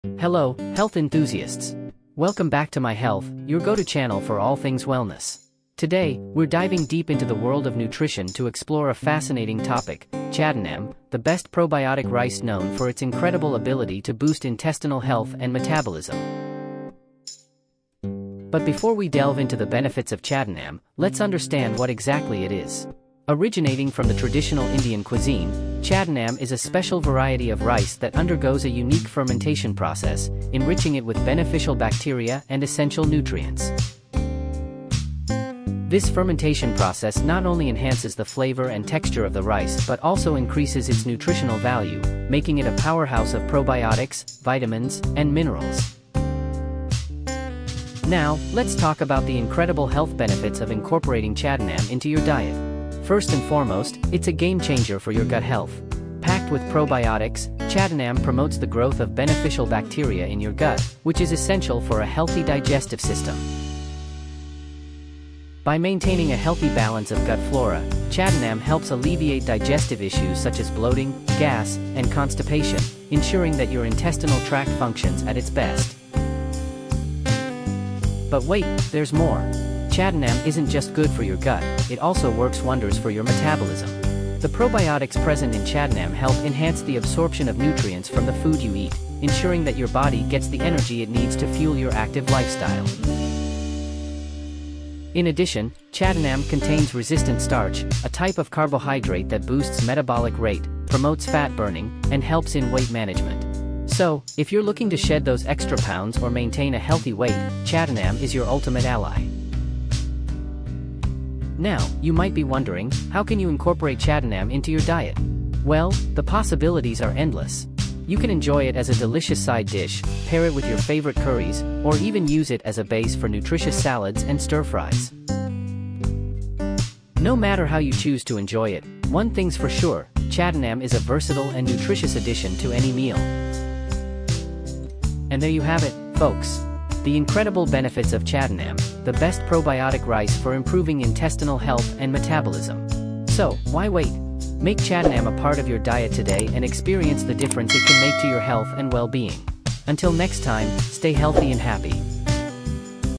Unlocking the Power of Chaddannam The Ultimate Probiotic Rice for Enhanced Intestinal Health and Met
**Unlocking the Power of Chaddannam**: The Ultimate Probiotic Rice for Enhanced Intestinal Health and Metabolism.

Dive into the fascinating world of Chaddannam with us today on My Health! Known as a superb probiotic rice, Chaddannam originates from traditional Indian cuisine and is transforming gut health and metabolism with every serving. 🌾✨

In this video, we uncover the secrets behind Chaddannam's unique fermentation process that not only enhances its flavor but also significantly boosts its nutritional value, making it rich in probiotics, vitamins, and essential minerals. Discover how this special variety of rice can be a game-changer for your digestive health, helping to balance gut flora and alleviate common digestive issues.

But that's not all—Chaddannam is also a champion for your metabolism! With components like resistant starch, it promotes a higher metabolic rate, aids in fat burning, and assists in effective weight management. Whether you're looking to shed some pounds or simply maintain a healthy weight, Chaddannam could be your best ally.

Wondering how to integrate Chaddannam into your meals? We'll show you how versatile and delicious it can be, whether as a side dish, paired with curries, or used as a base for nutritious salads and stir-fries.

Join us as we explore the incredible health benefits of Chaddannam, and learn why making it a part of your diet can greatly enhance your health and well-being. Stay healthy and happy! 🍚🌿